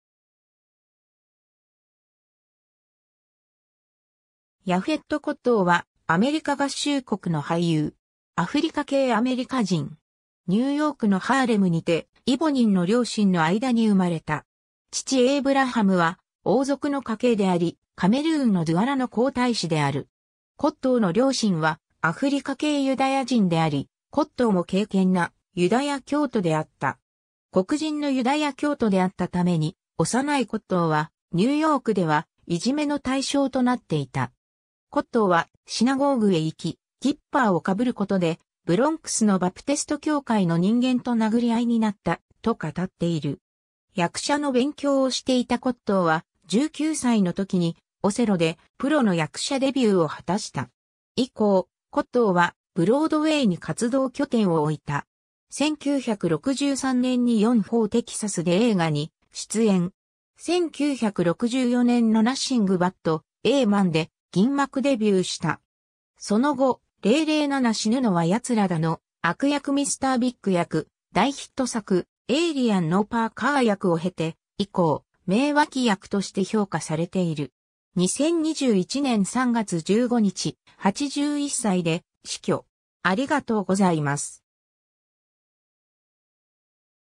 ヤフェット・コットー
ヤフェット・コットー（Yaphet Kotto, 1939年11月15日 - 2021年3月15日）は、アメリカ合衆国の俳優。
 ニューヨークのハーレムにてイボ人の両親の間に生まれた。
父エイブラハムは王族の家系であり、カメルーンのドゥアラの皇太子である。
 コットーの両親はアフリカ系ユダヤ人であり、コットーも敬虔なユダヤ教徒であった。
黒人のユダヤ教徒であったために、幼いコットーはニューヨークではいじめの対象となっていた。
コットーは「シナゴーグへ行き、キッパーをかぶることでブロンクスのバプテスト教会の人間と殴り合いになった」と語っている。
 役者の勉強をしていたコットーは、19歳のときに『オセロ』でプロの役者デビューを果たした。
以降、コットーはブロードウェイに活動拠点を置いた。
 1963年に "4 For Texas"で映画に出演（クレジットなし）。
1964年の"Nothing But a Man"で銀幕デビューした。
その後、『007 死ぬのは奴らだ』の悪役Mr.ビッグ役、大ヒット作『エイリアン』のパーカー役を経て、以降、名脇役として評価されている。
 2021年3月15日、81歳で死去 。